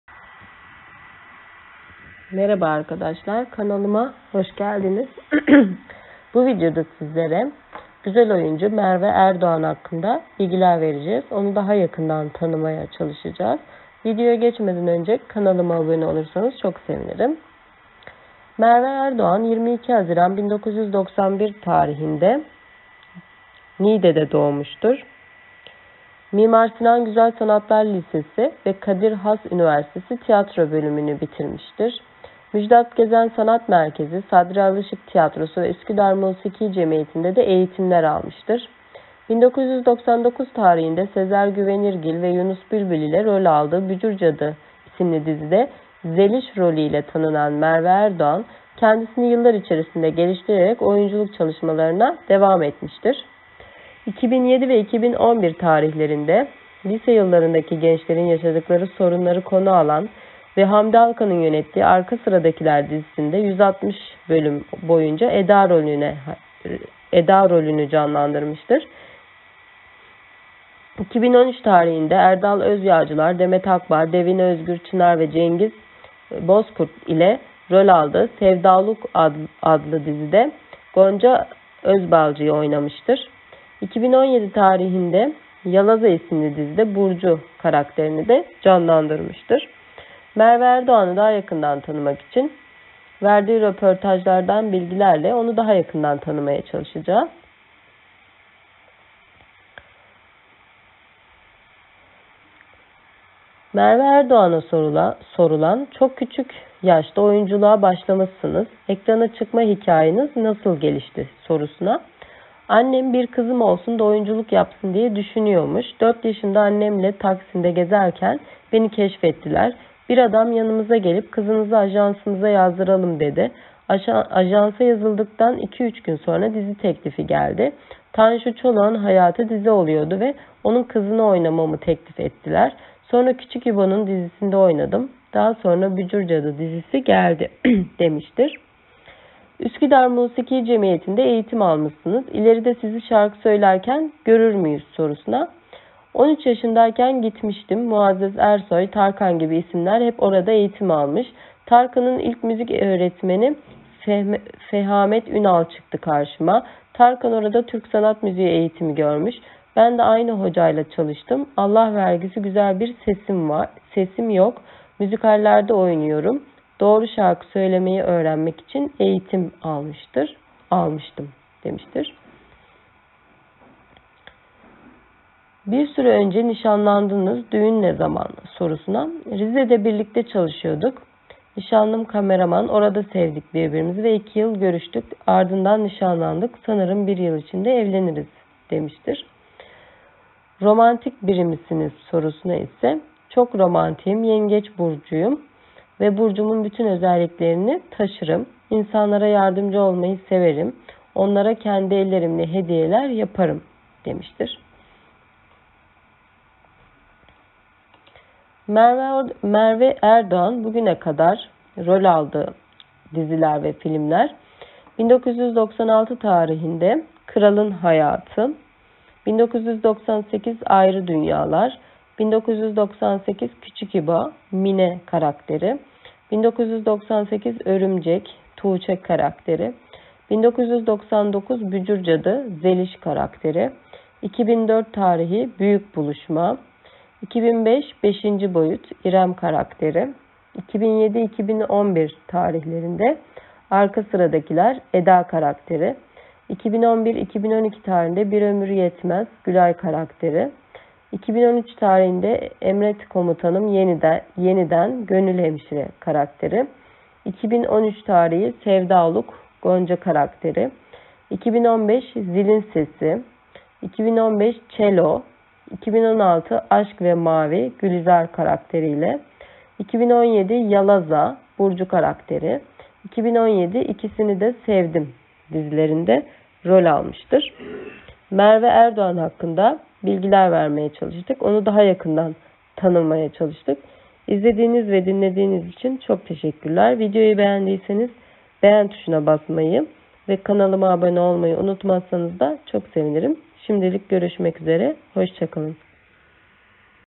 Güzel Oyuncu Merve Erdoğan
Merve Erdoğan 1991 tarihinde Niğde'de doğmuştur. Bücür Cadı dizisinde küçük yaşlarda oyunculukla tanışmıştır. Özel Moda Mimar Sinan Güzel Sanatlar lisesinde tiyatro eğitimi görmüştür. Kadir Has Üniversitesi tiyatro bölümünü bitirmiştir. Eğitimine önem veren oyuncu Müjdat Gezen Sanat Merkezi ve Sadri Alışık Tiyatrosundan eğitimler almıştır.

Merve Erdogan was born in Nigde in 1991. She met acting at an early age in The Little Witch. He studied theatre at Private Fashion Mimar Sinan Fine Arts High School. He graduated from Kadir Has University, department of theatre. The actress, who cares about her education, has received trainings from Müjdat Gezen Art Center and Sadri Alışık Theatre.